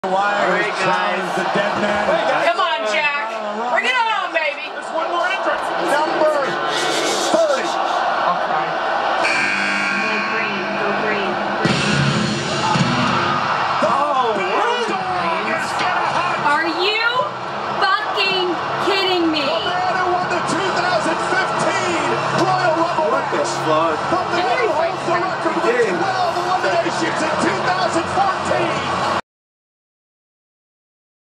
Live Royal Rumble Reaction to Roman Reigns (2017)
Our live reaction when we found out Roman Reigns was number 30 in the Royal Rumble.